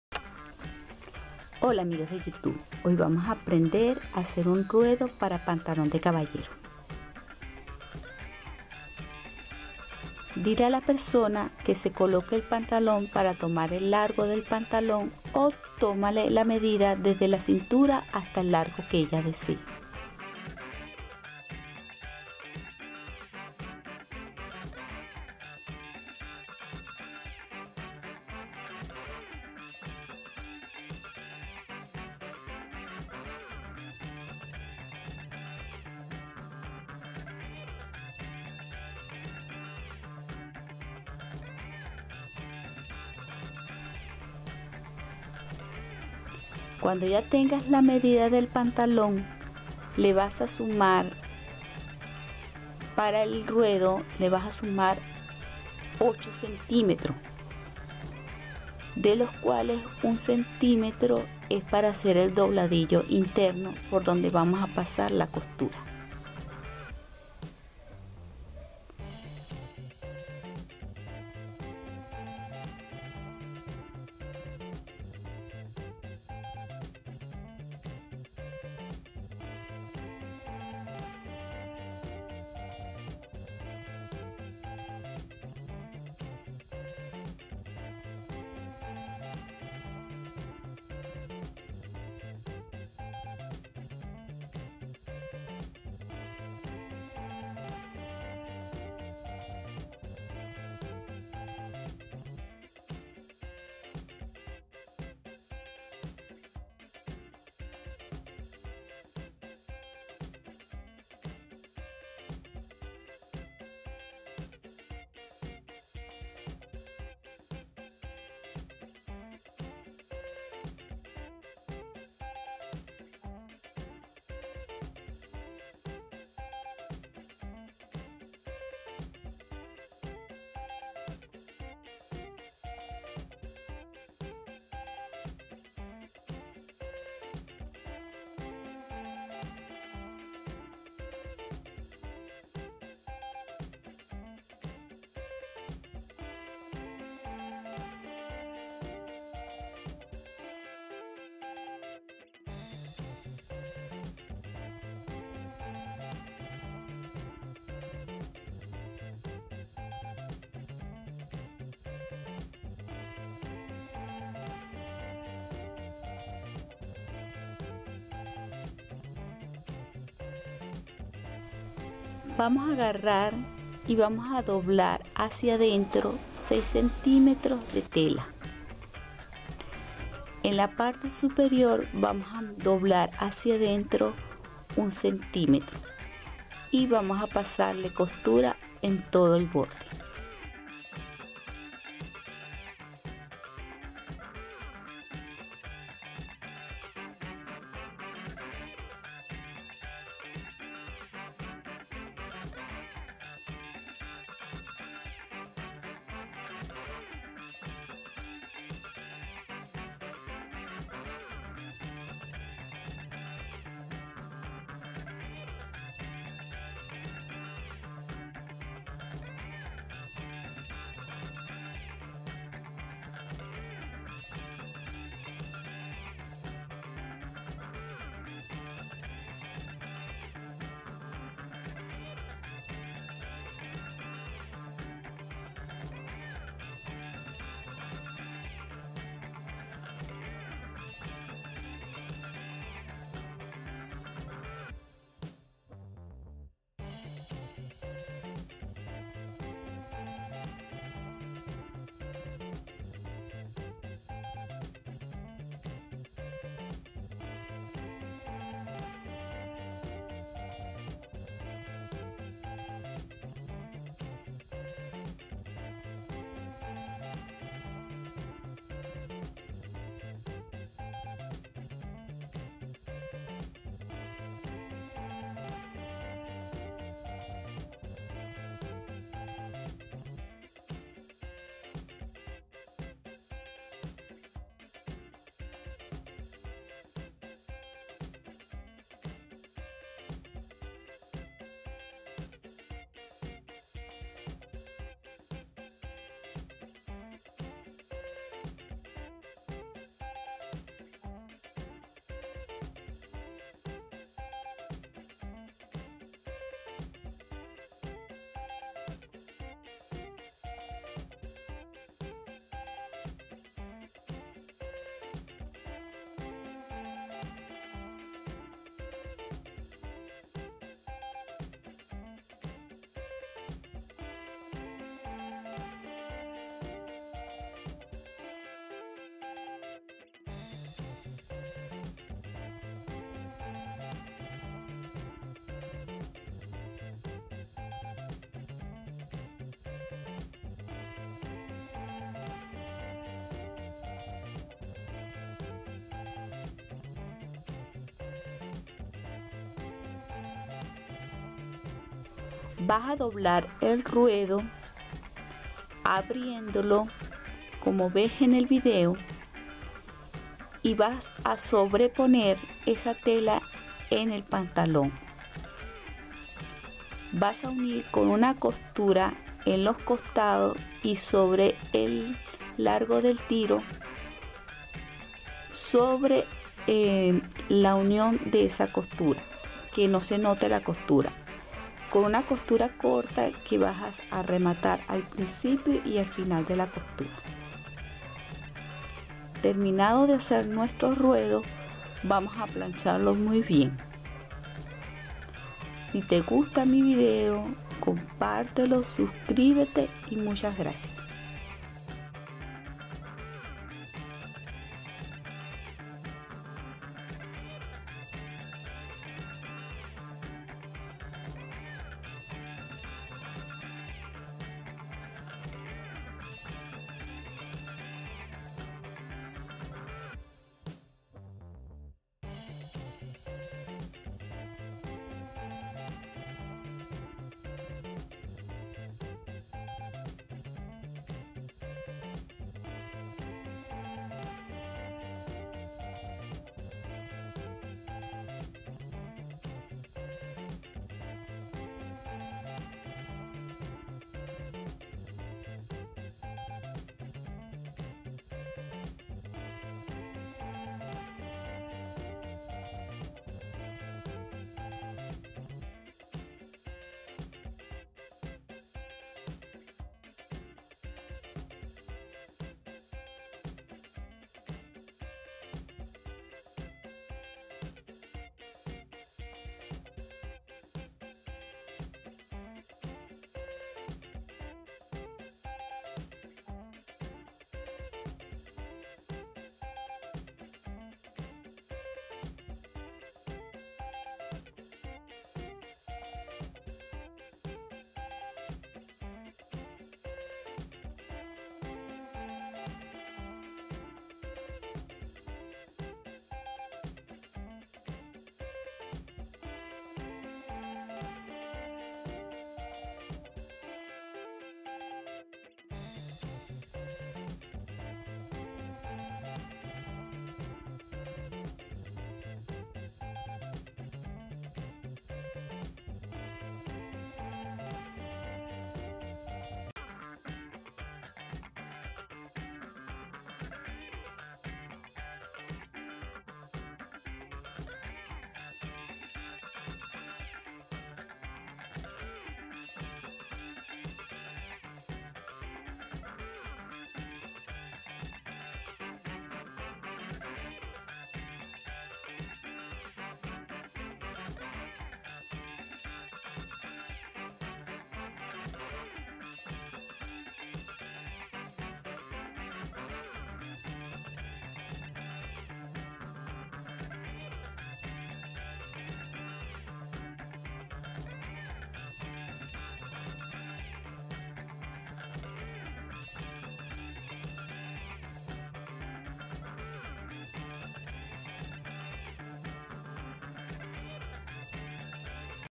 Ruedo de pantalón. Como hacer un ruedo a los pantalones de caballero
hola amigas quiero enseñarles a coser este ruedo de pantalón en simples pasos.